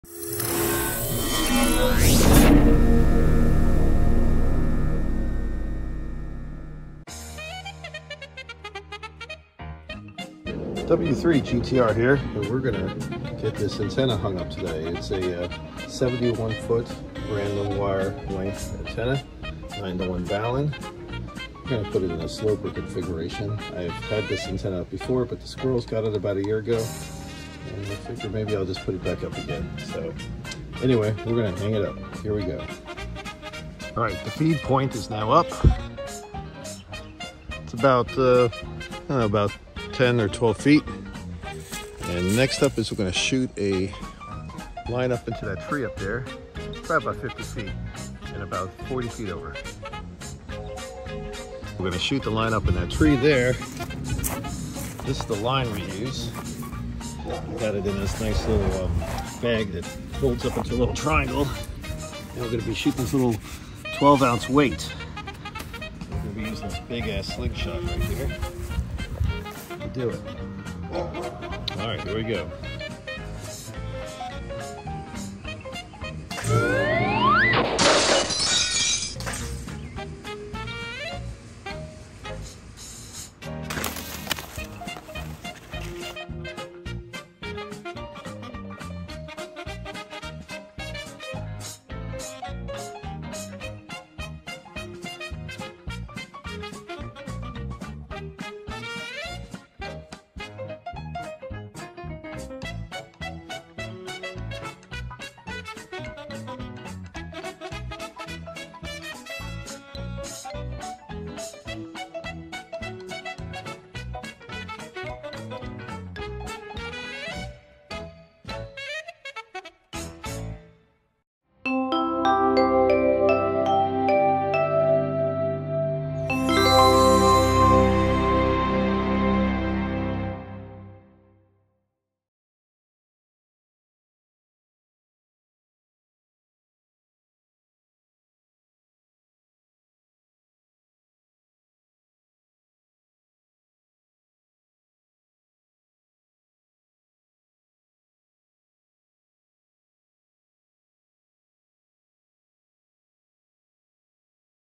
Random Wire 71-foot Slopper
My install and testing of a random wire 71-foot antenna configured as a slopper.